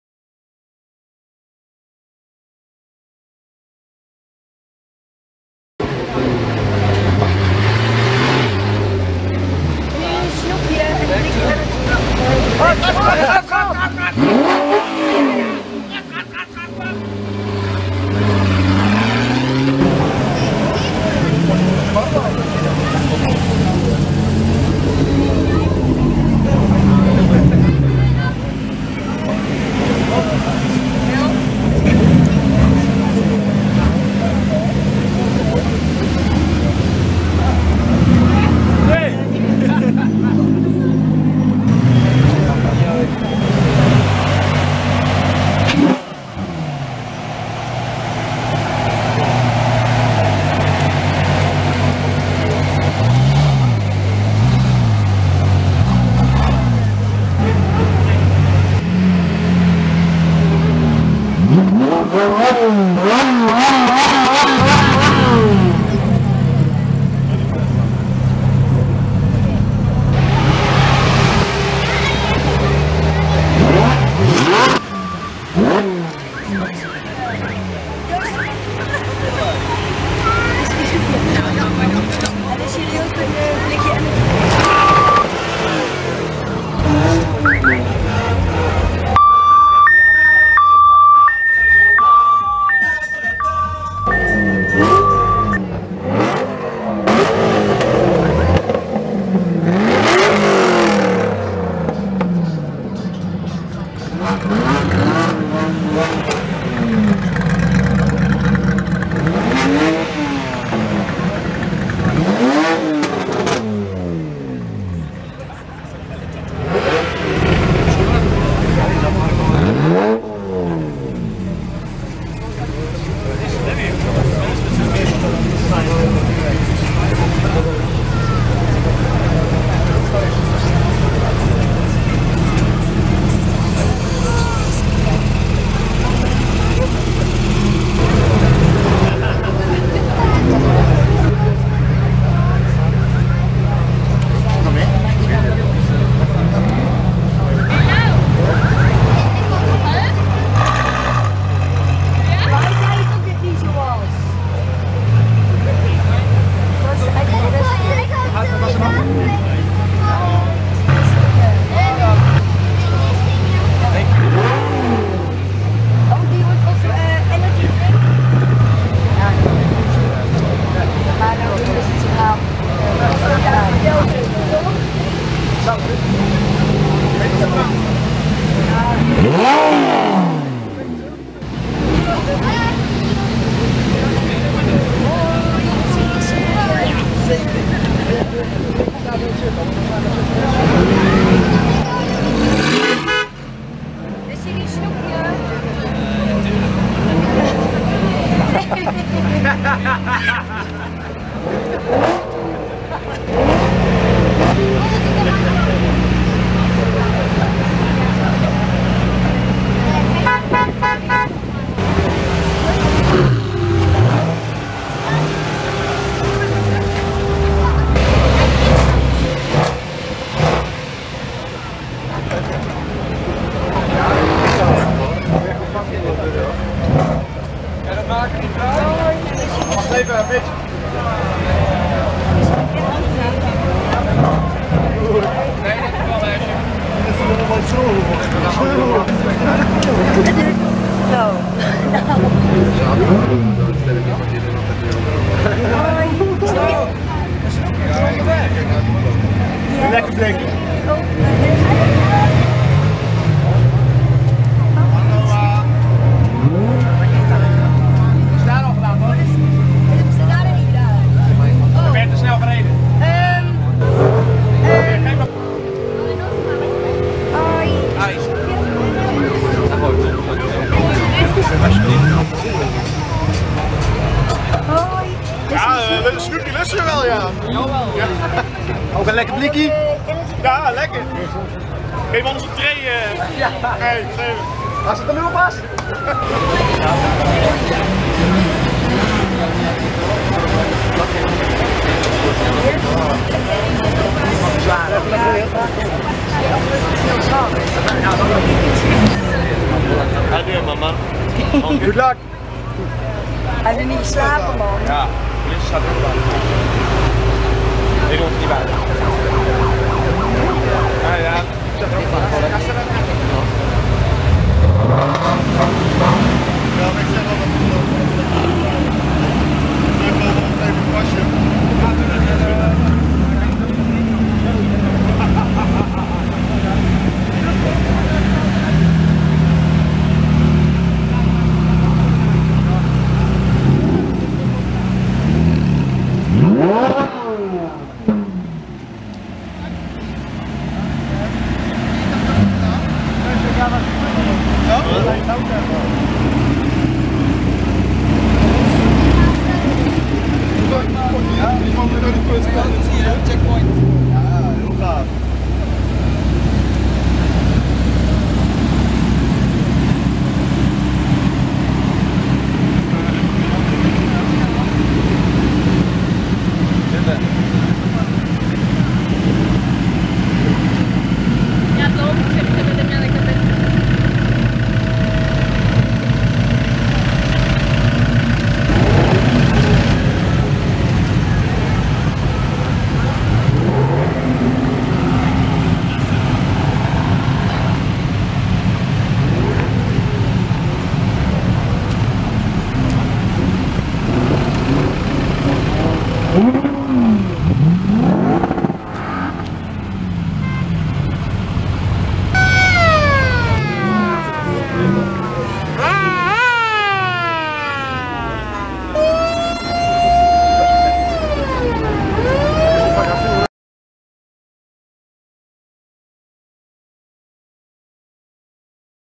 Diamond Race 2016 Brno Bulvar24
Diamond Race 2016 - Checkpoint v Brně u OC Olympie